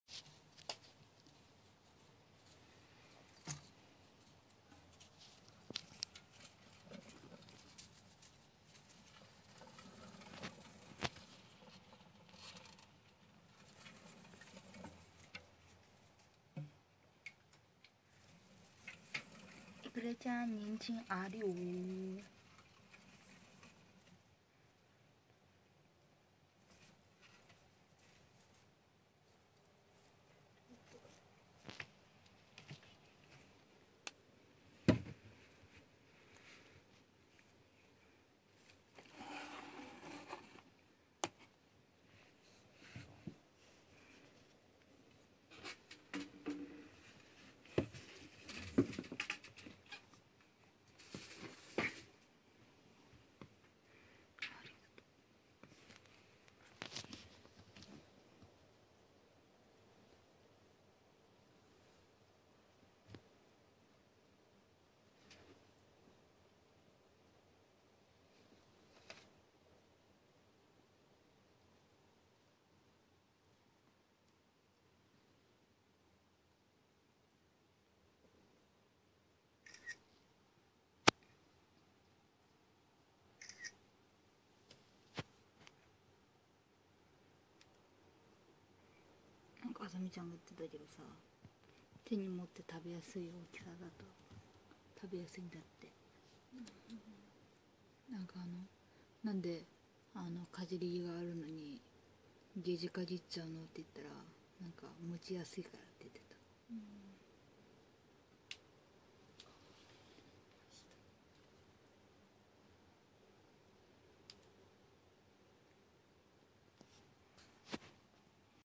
エサはすぐ食べないけどニンジンはすぐ食べるハムスター Golden hamster
やっぱりニンジン大好きなエクレアちゃん！
●エクレアちゃんチャンネル

●エクレアちゃんブログ（アメーバブログ）